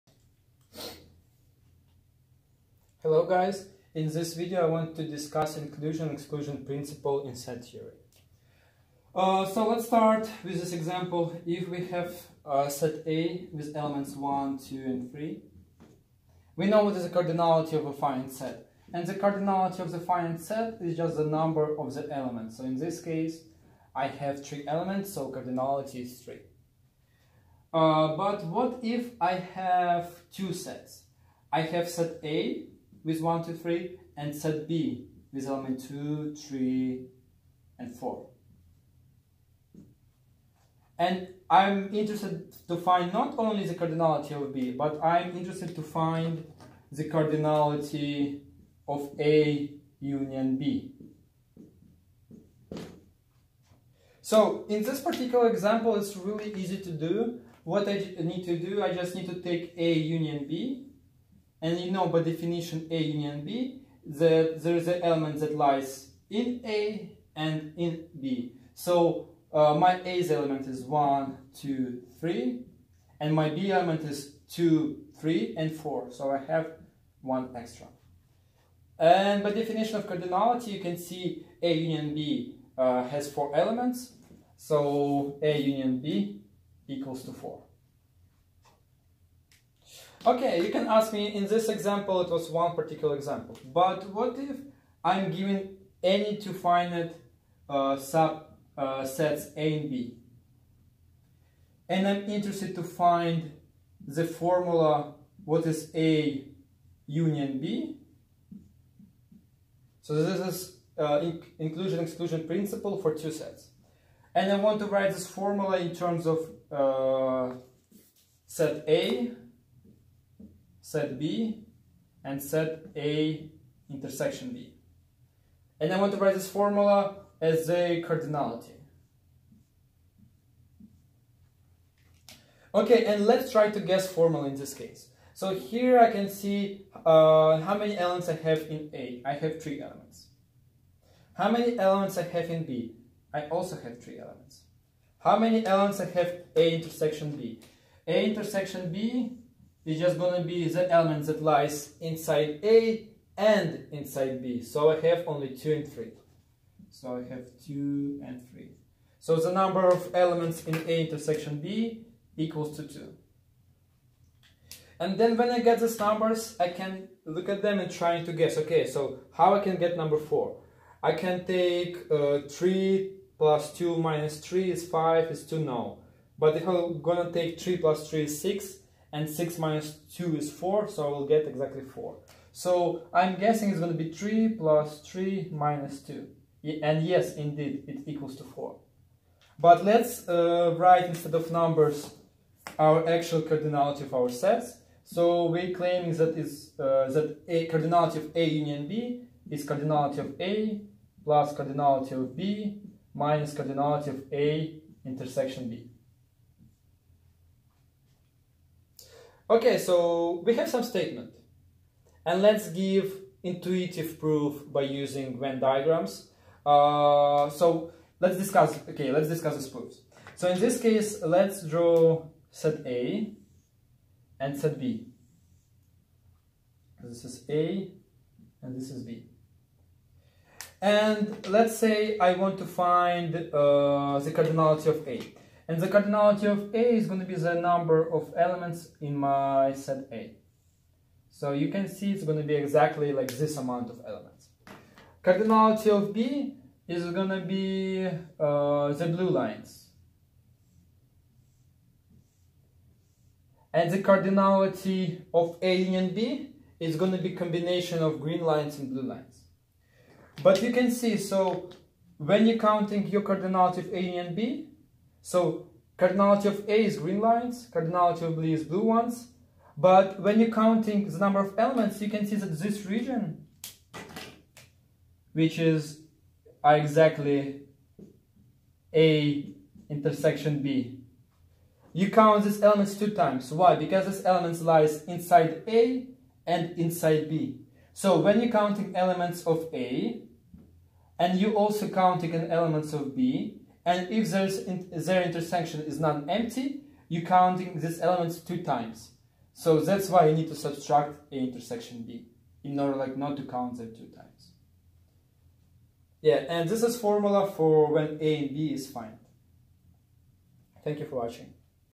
Set Theory. Inclusion-exclusion Principle.
In this video, we are going to discuss Inclusion-exclusion Principle for two finite sets.